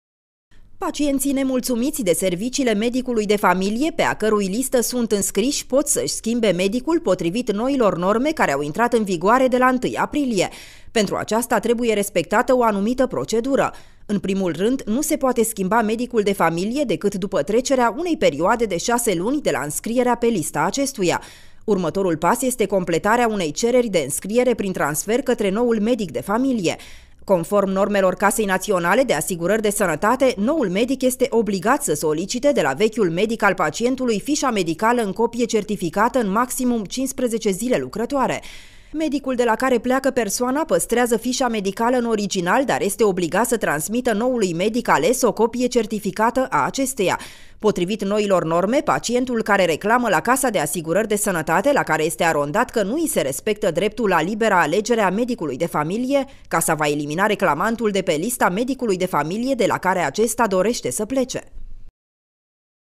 Medicul de familie, la alegere
Pacienții care doresc să își schimbe medicul de familie pot face acest lucru, dar după trecerea unei perioade de 6 luni de la înscrierea pe lista acestuia. La noul medic se completează o cerere de înscriere prin transfer, după care acesta va solicita de la vechiul medic fișa medicală în copie certificată.

     Pacienții nemulțumiți de serviciile medicului de familie, pe a cărui listă sunt înscriși, pot să își schimbe medicul, potrivit noilor norme care au intrat în vigoare de la 1 aprilie. Pentru aceasta trebuie respectată o anumită procedură. În primul rând, nu se poate schimba medicul de familie decât după trecerea unei perioade de 6 luni de la înscrierea pe lista acestuia. Următorul pas este completarea unei cereri de înscriere prin transfer către noul medic de familie. Conform normelor Casei Naționale de Asigurări de Sănătate, noul medic este obligat să solicite de la vechiul medic al pacientului fișa medicală în copie certificată, în maximum 15 zile lucrătoare. Medicul de la care pleacă persoana păstrează fișa medicală în original, dar este obligat să transmită noului medic ales o copie certificată a acesteia. Potrivit noilor norme, pacientul care reclamă la Casa de Asigurări de Sănătate la care este arondat că nu i se respectă dreptul la libera alegere a medicului de familie, Casa va elimina reclamantul de pe lista medicului de familie de la care acesta dorește sa plece.